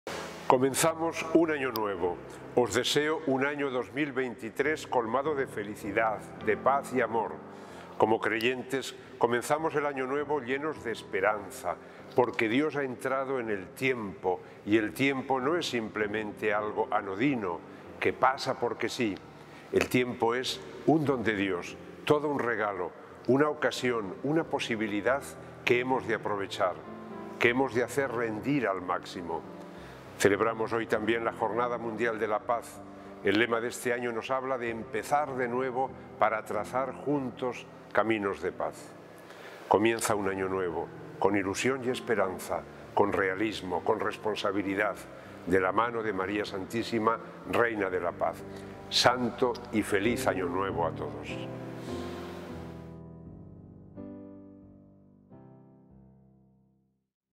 El Arzobispo de Sevilla felicita el Nuevo Año a la Archidiócesis- Diciembre 2022
Monseñor José Ángel Saiz Meneses, arzobispo de Sevilla, ofrece sus mejores deseos a los fieles de la Archidiócesis de Sevilla con motivo del nuevo año 2023.

"Os deseo un año 2023 colmado de felicidad, de paz y amor. Como creyentes, comenzamos el año nuevo llenos de esperanza".